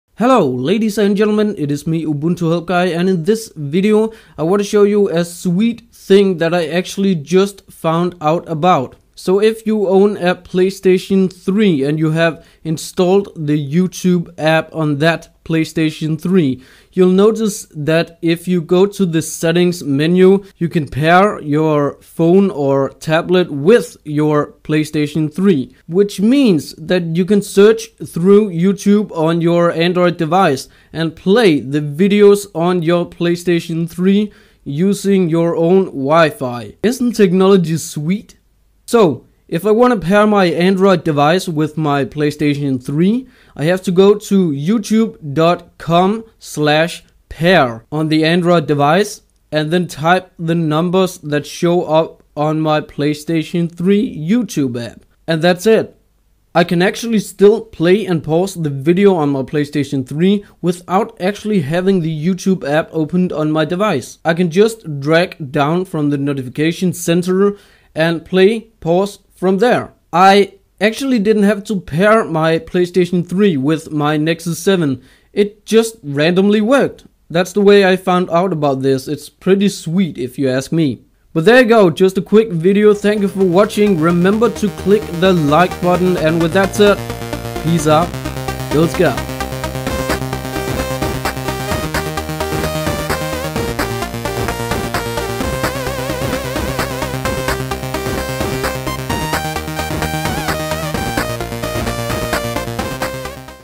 Connect Android Device to PS3 YouTube App
In this video, I'm gonna show you how to connect your Android device to the PS3 YouTube app via your home wifi! Remember to like the video if you enjoyed!